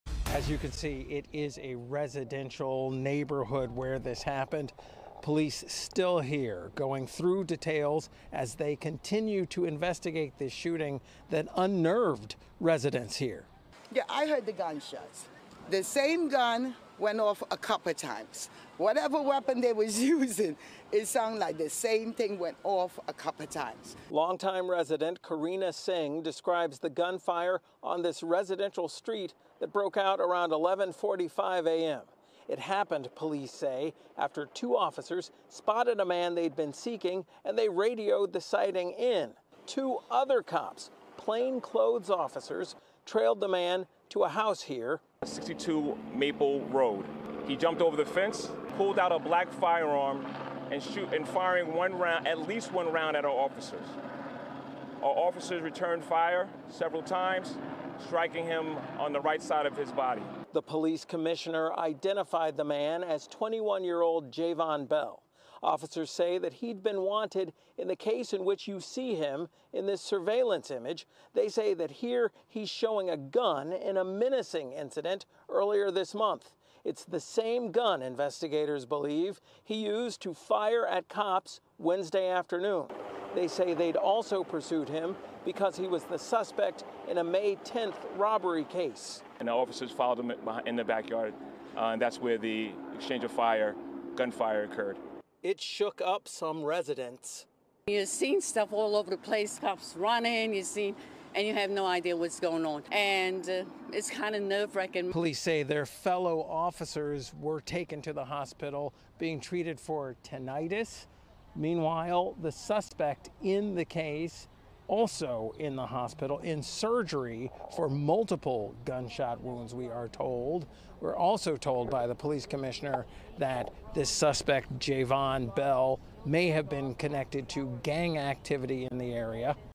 Man, 21, exchanged gunfire with cops: officials
The shooting happened in a residential neighborhood in this Suffolk County, Long Island town, at around 11:45 a.m. The suspect, who Suffolk County Police Commissioner Rodney Harrison identified as Jayvon Bell, 21, tried to evade officers by entering somebody's backyard, the commissioner said.